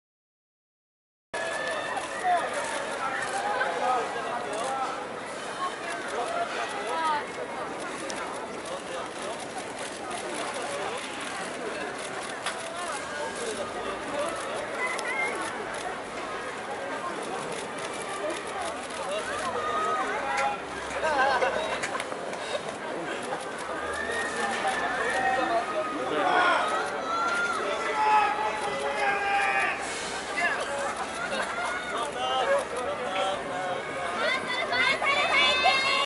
[뉴스인스타] 171117 더 유닛B 빨강조 KBS 뮤직뱅크 출근길
171117 더 유닛B 빨강조 뮤직뱅크 리허설 출근길 풍경 입니다.

GATE 9, VAV, 빅톤(VICTON), Wanna One, 구구단, 닉앤쌔미, 더 유닛, 러블리즈, 레드벨벳, 마스크, 몬스타엑스, 사무엘, 세븐틴, 소나무, 아스트로, 트와이스가 참여했습니다.